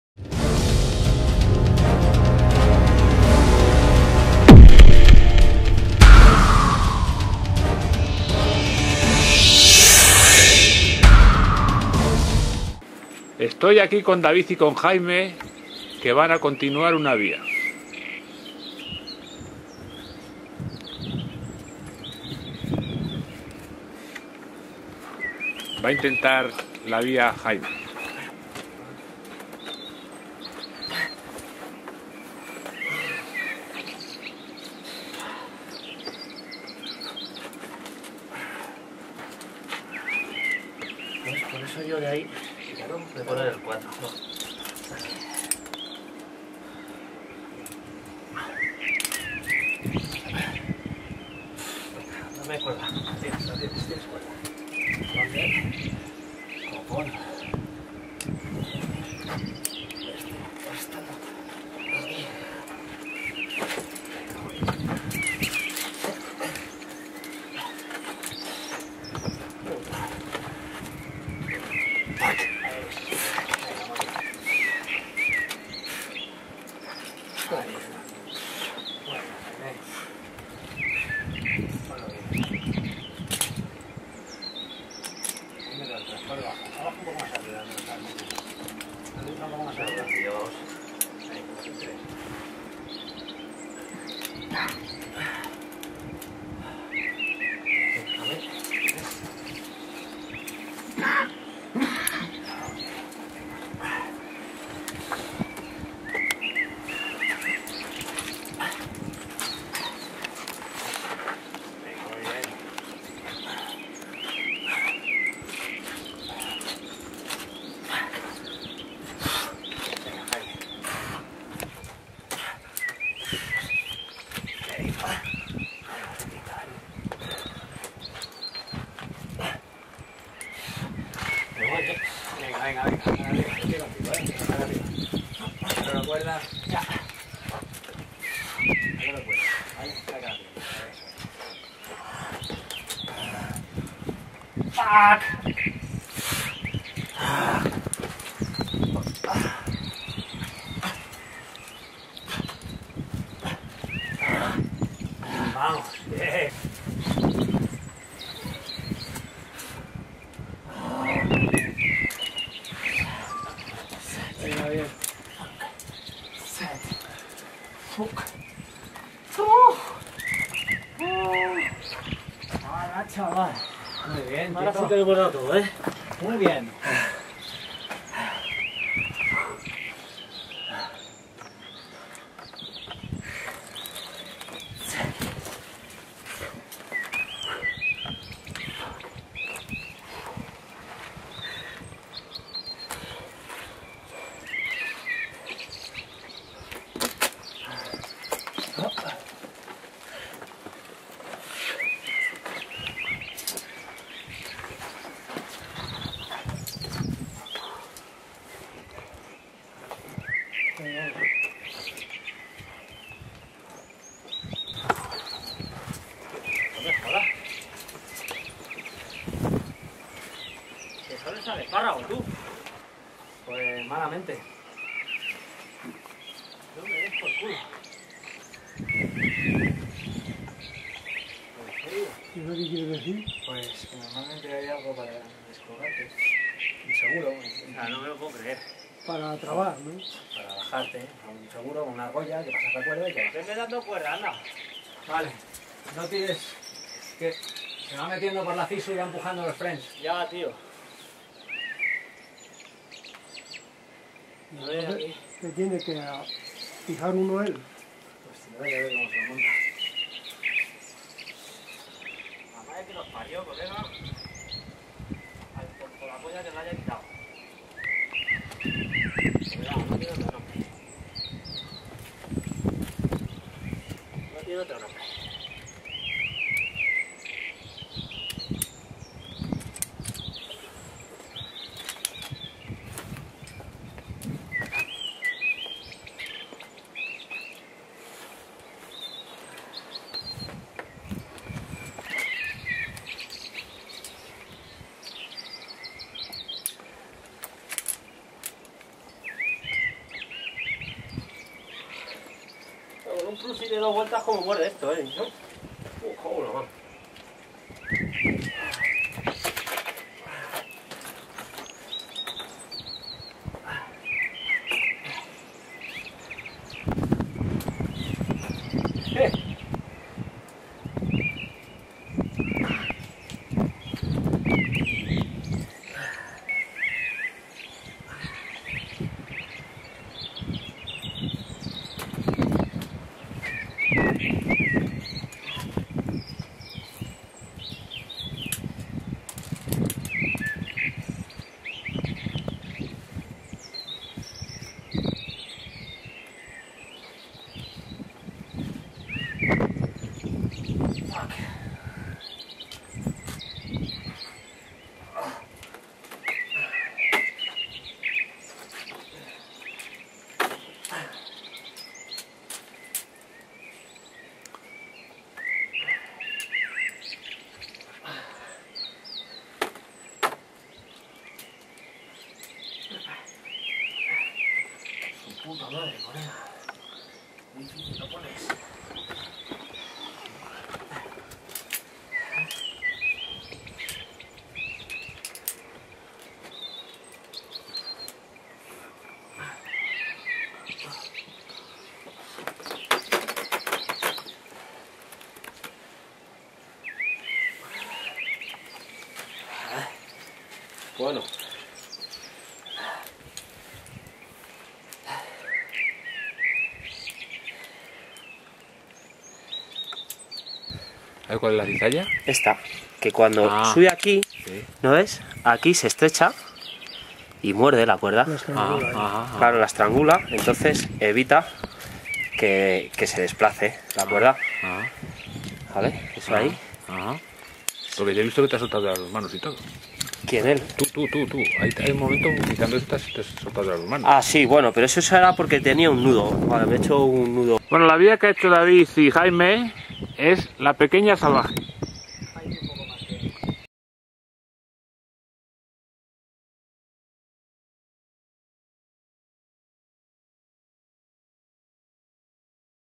La Pedriza escalada de Jaime a la "Pequeña Salvaje"
Para ver el Documental integro ir a En esta ocasión vamos a ver a Jaime y David haciendo la via de la Pequeña Salvaje.